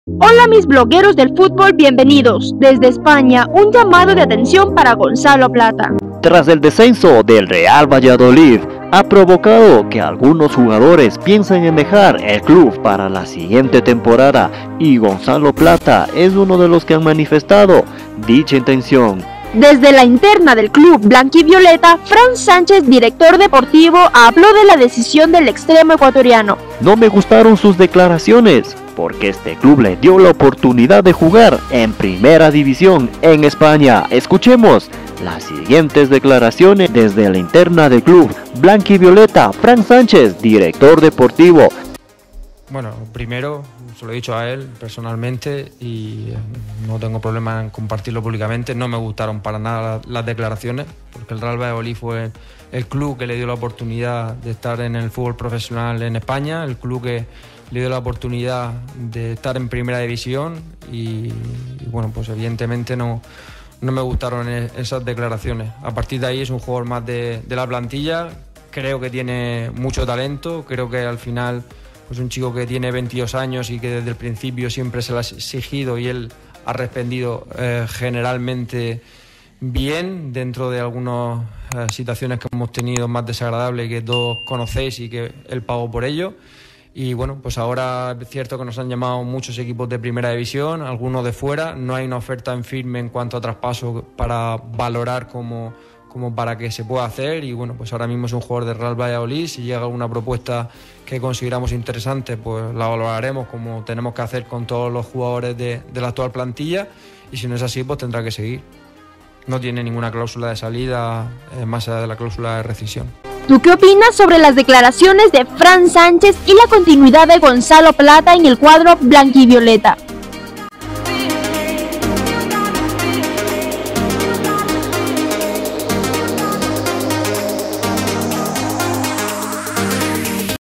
🇪🇸 "REGAÑO" a GONZALO PLATA desde ESPAÑA MIRA LO QUE DIJERON
REGAÑO a GONZALO PLATA desde el Real Valladolid de ESPAÑA tras sus declaraciones por querer dejar dicho Club.
Gonzalo Plata Real Valladolid
Gonzalo Plata Hoy